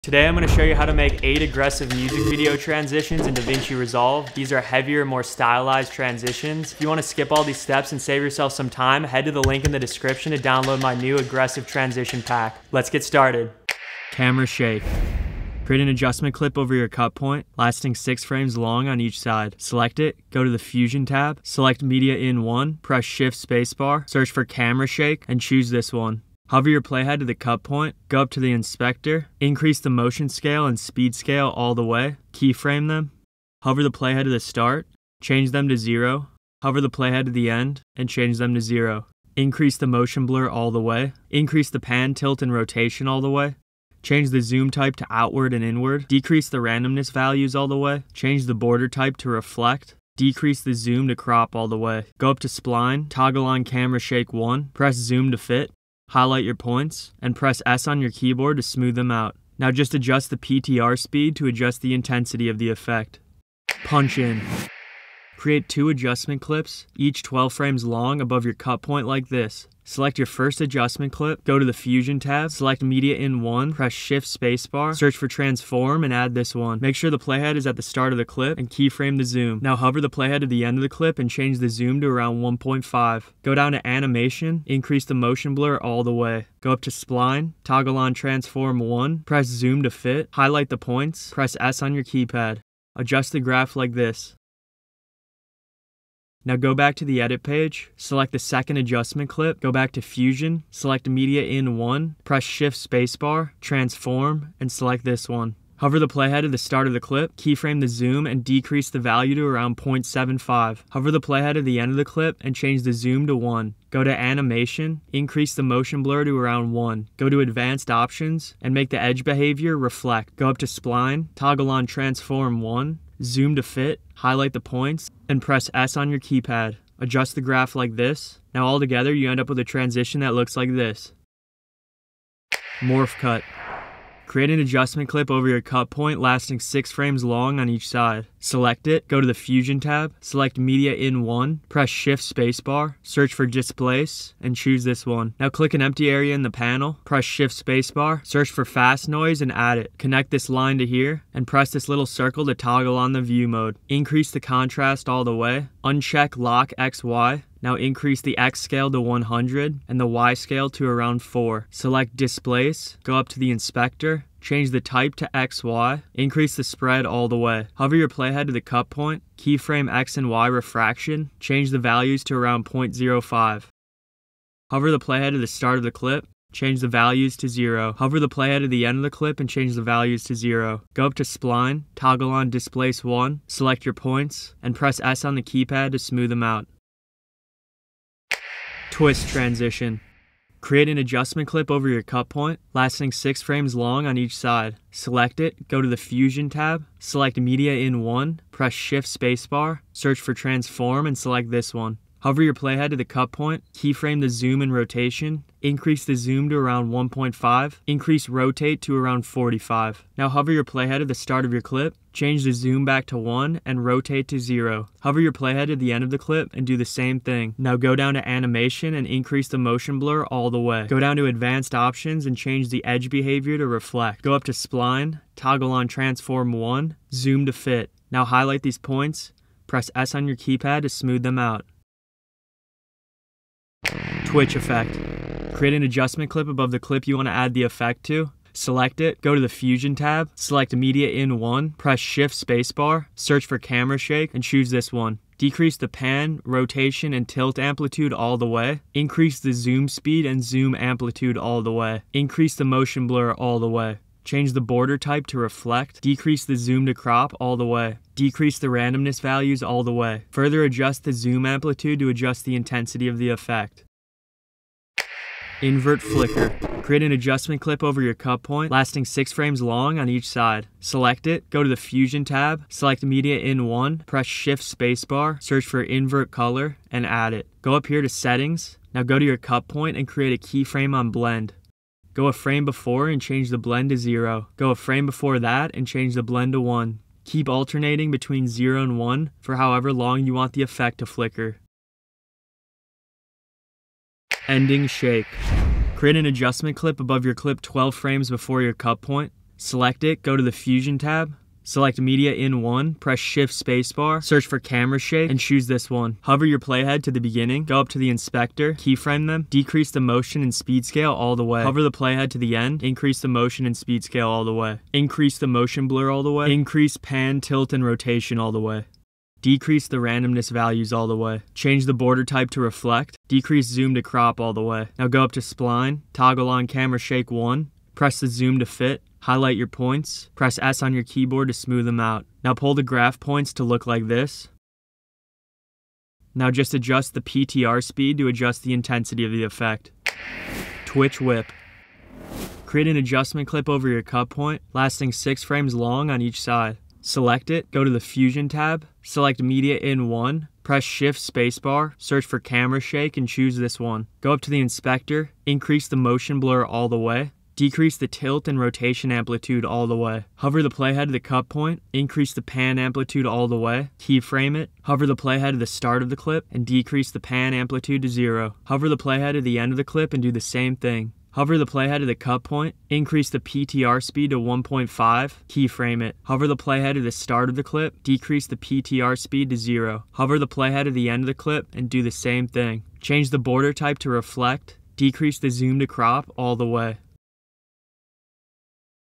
8 Aggressive Transitions in DaVinci Resolve
Music video transitions for DaVinci Resolve
Music video effects in DaVinci Resolve

Chapters:
0:00 Intro
0:14 Camera Shake
1:04 Punch In
2:13 Morph Cut
3:09 Twist Cut
3:55 Twitch Effect
4:25 Invert Flicker
4:55 Ending Shake
5:40 Twitch Whip